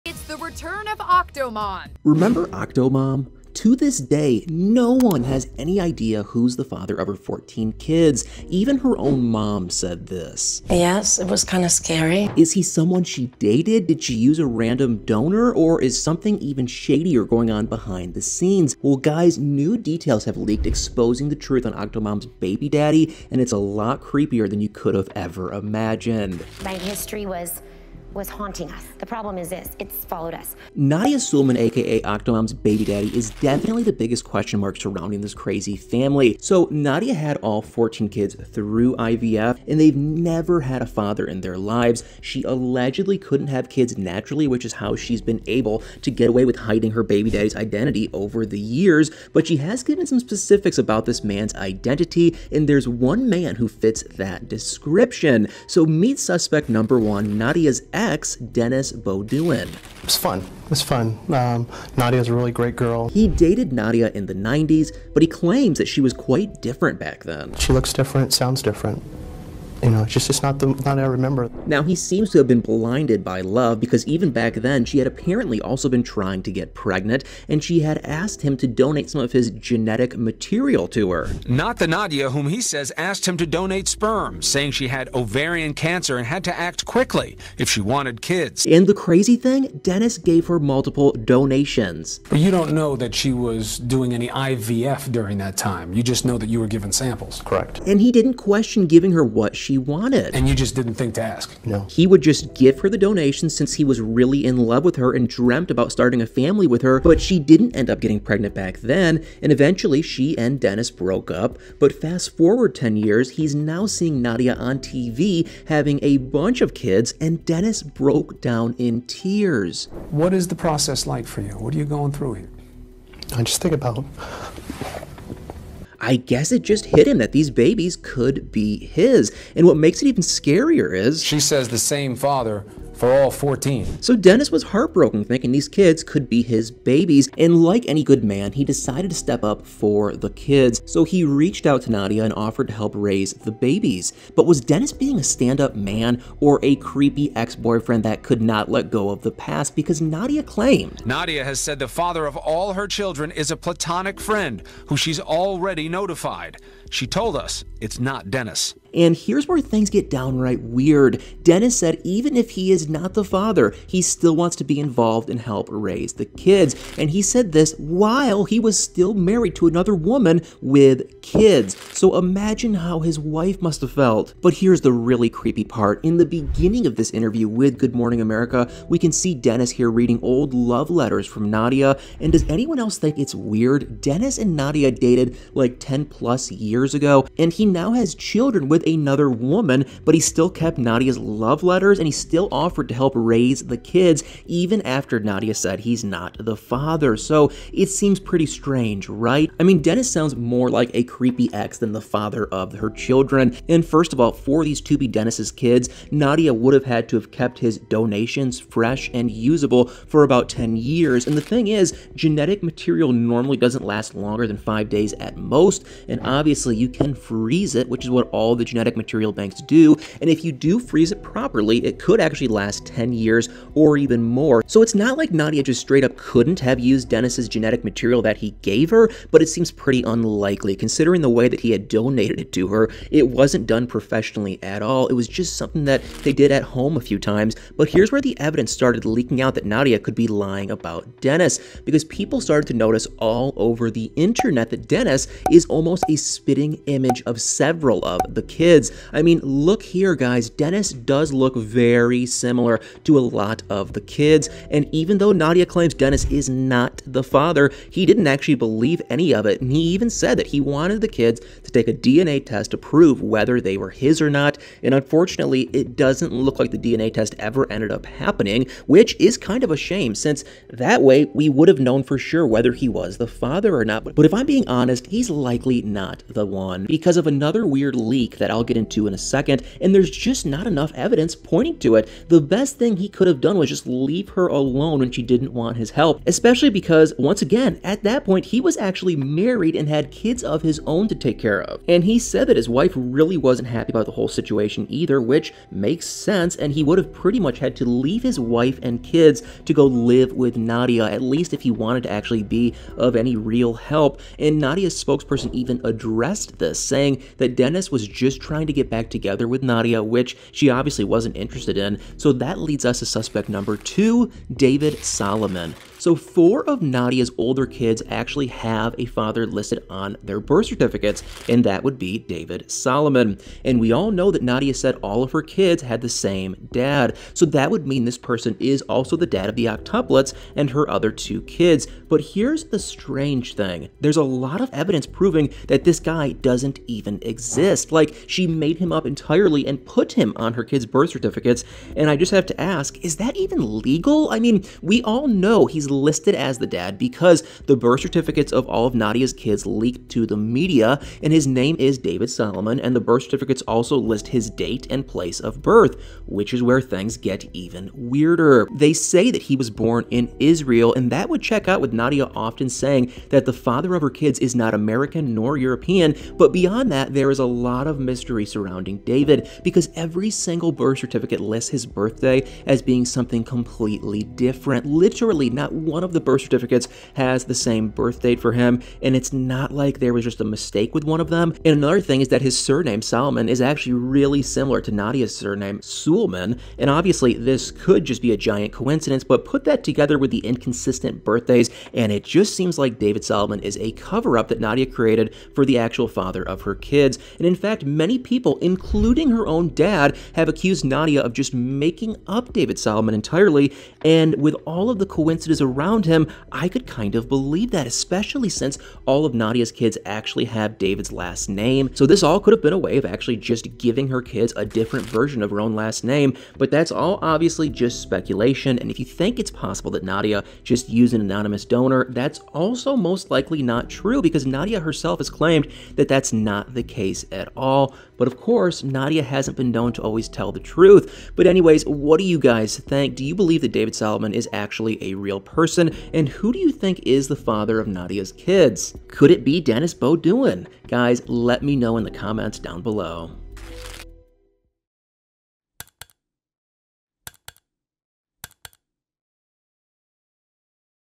Octomom's Weird Baby Daddy Update...
Remember Octomom? How could you forget the story :)

It's been years and we still have no idea who is the father of Octomom's (Nadya Suleman's) children.